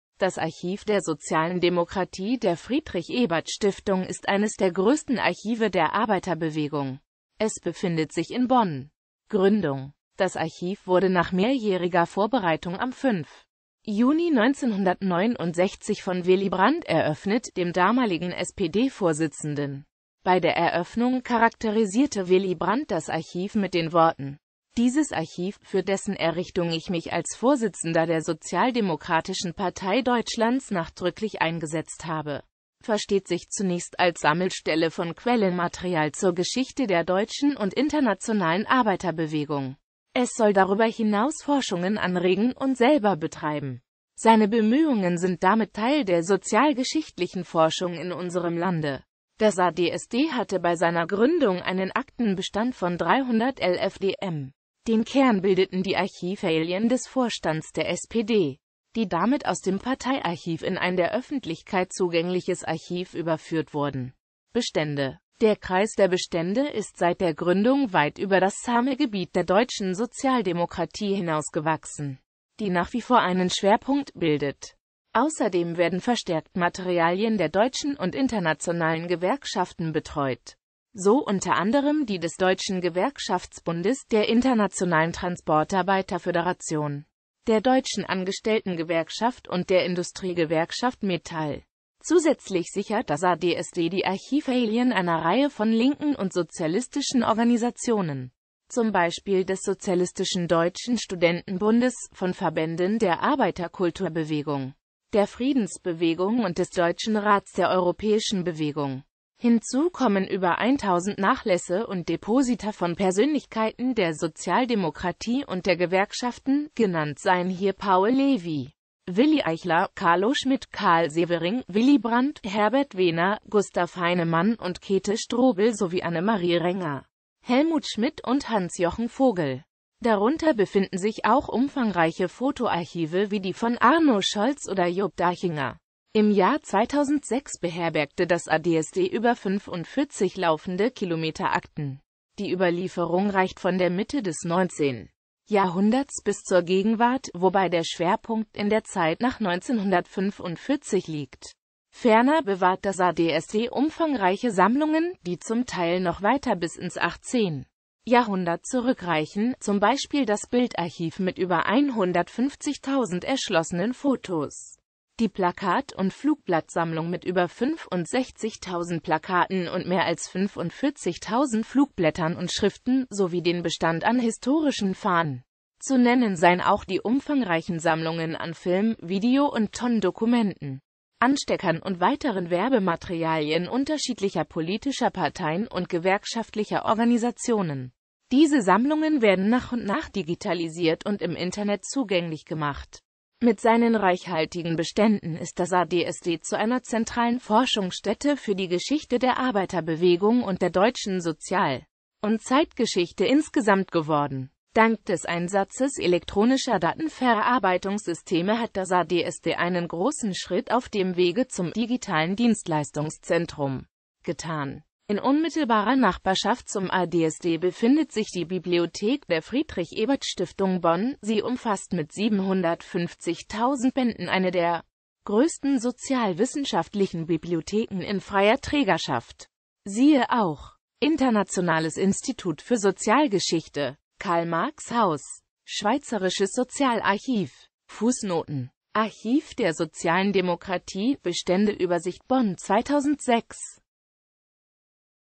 Archiv der sozialen Demokratie
Vielen Dank für Ihre Unterstützung:

Das Archiv der sozialen Demokratie (AdsD) der Friedrich-Ebert-Stiftung (FES) ist eines der größten Archive der Arbeiterbewegung.Es befindet sich in Bonn.

Bild-Copyright-Informationen
Urheber Info: Qualle